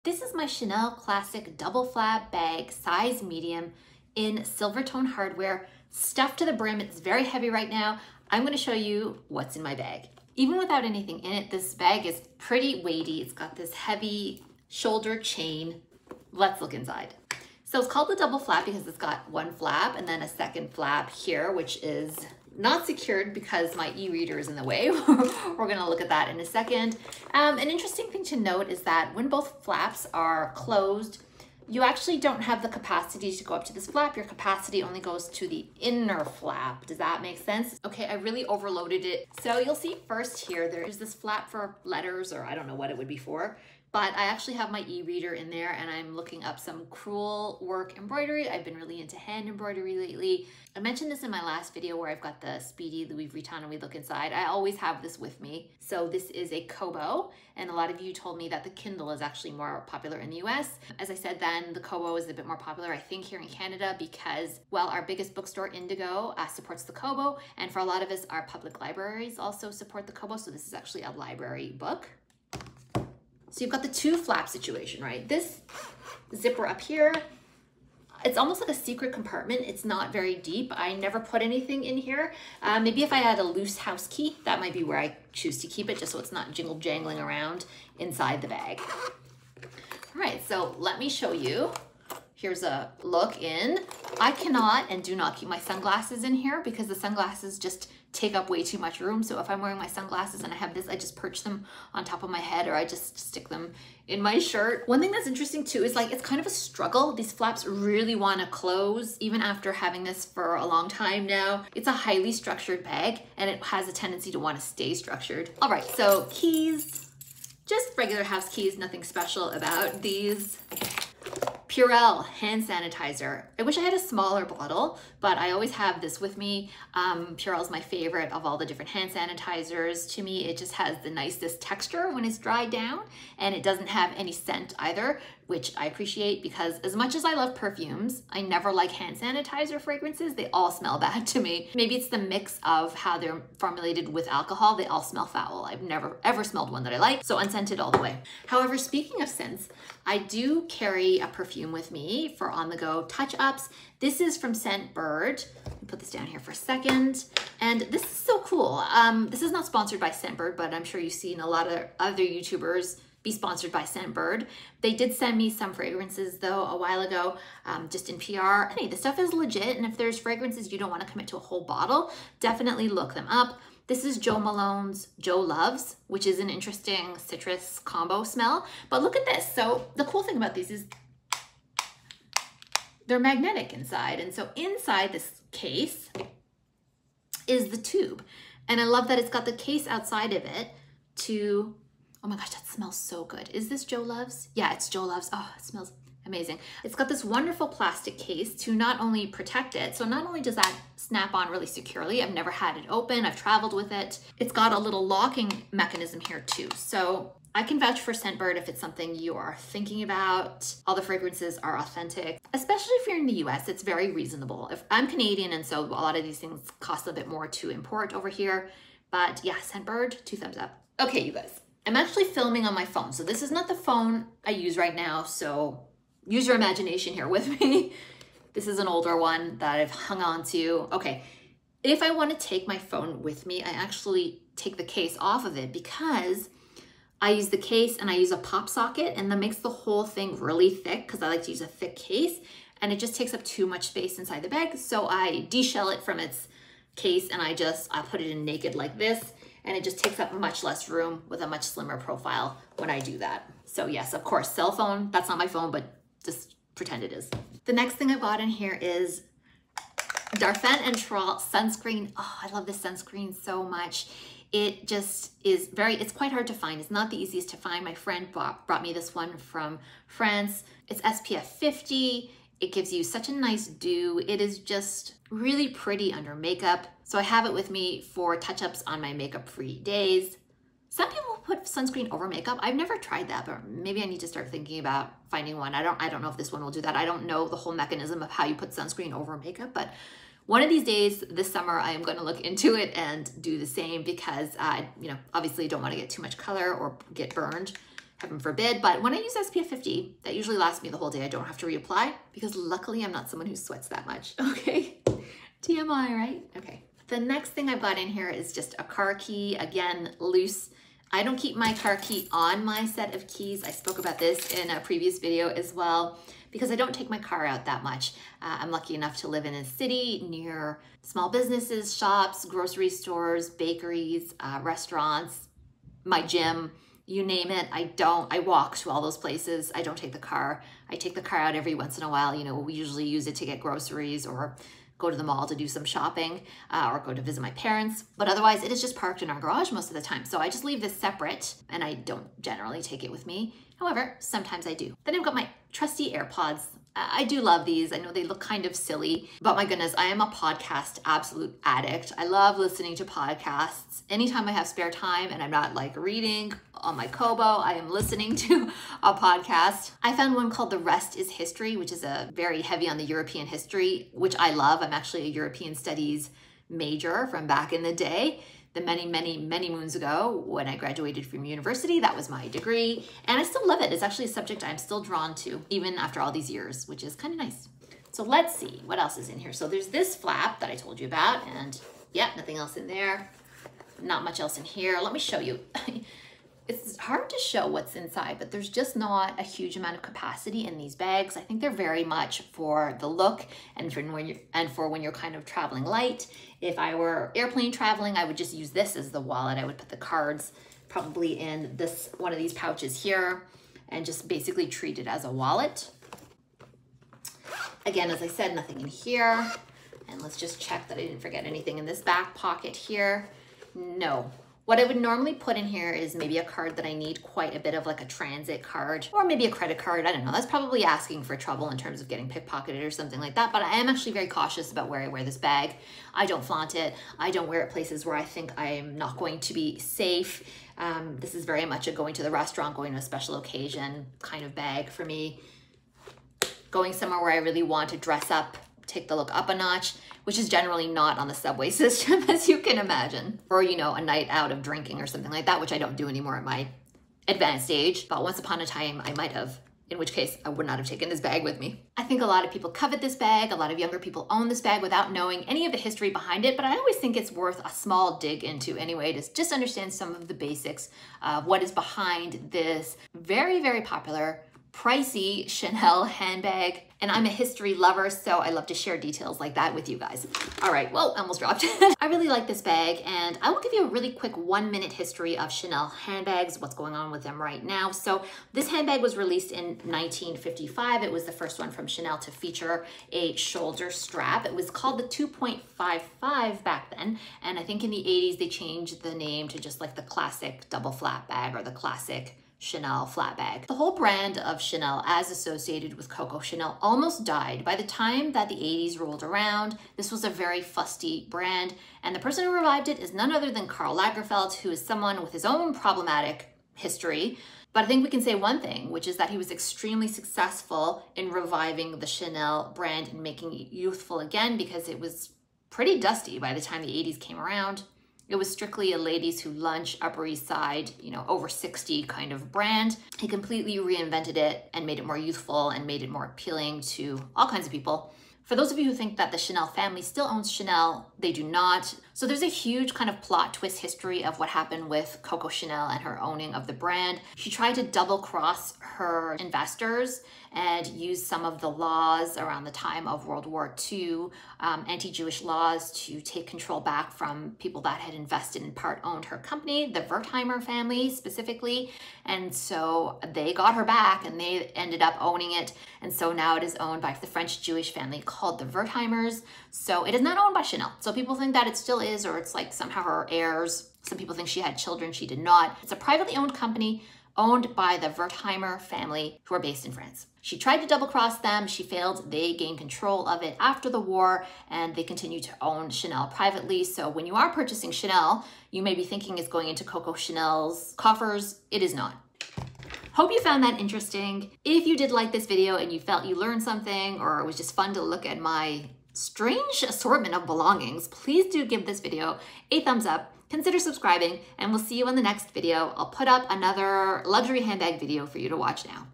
What's In My Bag? | Chanel Classic Double Flap Black Caviar Medium Bag
This is what's in my Chanel Classic Flap Bag...
Chanel Double Flap Caviar Silver Tone Hardware (PRELOVED), Same As Mine